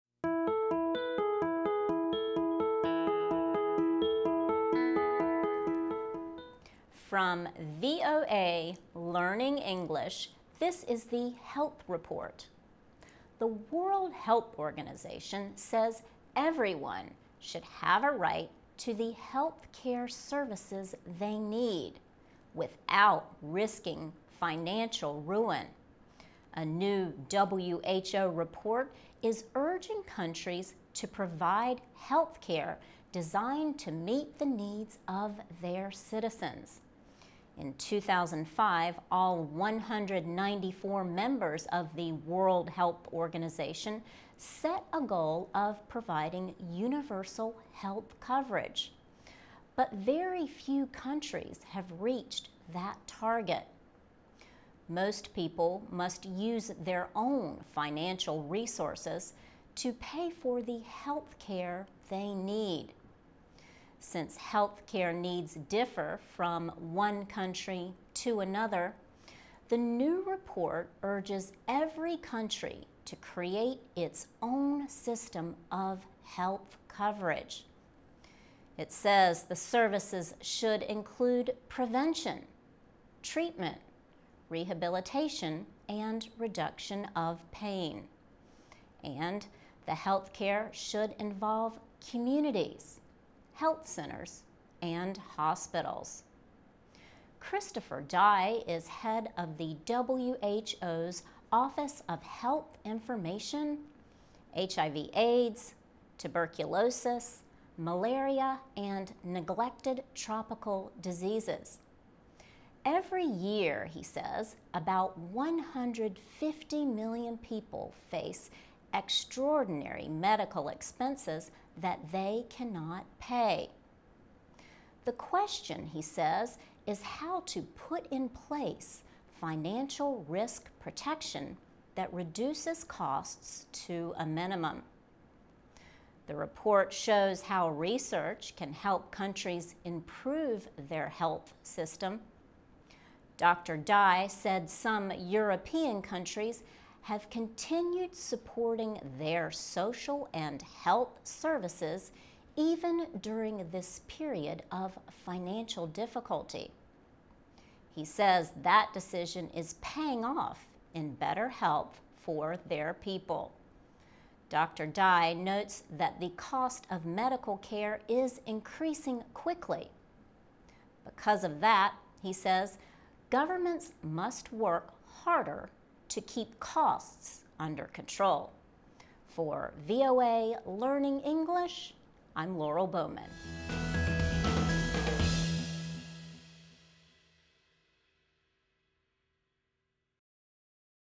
WHO Urges Health Care for All
From VOA Learning English, this is the Health Report.

The World Health Organization says everyone should have a right to the health care services they need without risking financial ruin. A new WHO report is urging countries to provide health care designed to meet the needs of their citizens. In 2005, all 194 members of the World Health Organization set a goal of providing universal health coverage. But very few countries have reached that target. Most people must use their own financial resources to pay for the health care they need.Since health care needs differ from one country to another, the new report urges every country to create its own system of health coverage. It says the services should include prevention, treatment, rehabilitation and reduction of pain. And the health care should involve communities, health centers and hospitals. Christopher Dye is head of the WHO's Office of Health Information, HIV/AIDS, Tuberculosis, Malaria and Neglected Tropical Diseases. Every year, he says, about 150 million people face extraordinary medical expenses that they cannot pay. The question, he says, is how to put in place financial risk protection that reduces costs to a minimum.The report shows how research can help countries improve their health system. Dr. Dye said some European countries have continued supporting their social and health services even during this period of financial difficulty. He says that decision is paying off in better health for their people. Dr. Dye notes that the cost of medical care is increasing quickly. Because of that, he says, governments must work harder to keep costs under control. For VOA Learning English, I'm Laurel Bowman.